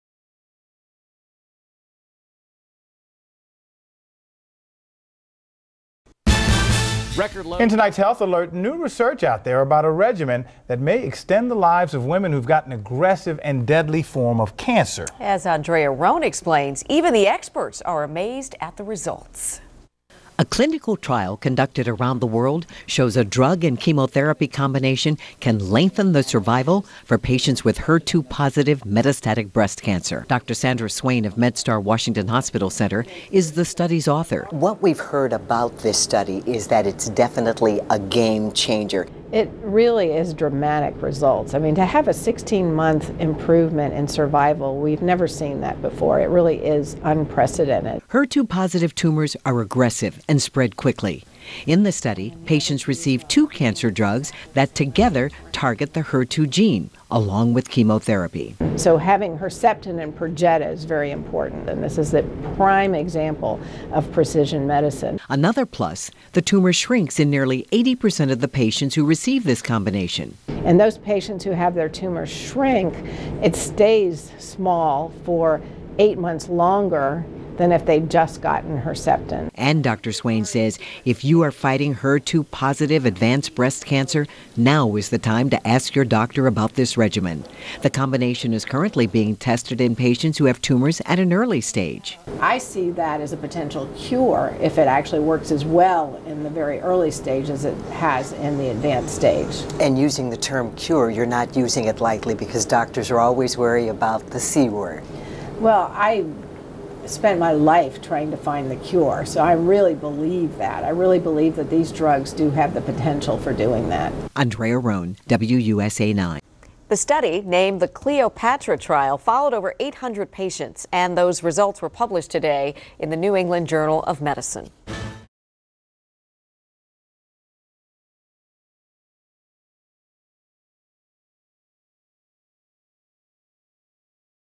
Sandra Swain, MD - CLEOPATRA Trial (WUSA 9)
A two-year long clinical trial conducted around the world shows a drug and chemotherapy combination can lengthen the survival for patients with HER2-positive metastatic breast cancer. The lead author of the CLEOPATRA study, Sandra Swain, MD, says the findings give patients with an aggressive disease hope to live a longer life.

Video courtesy of WUSA 9.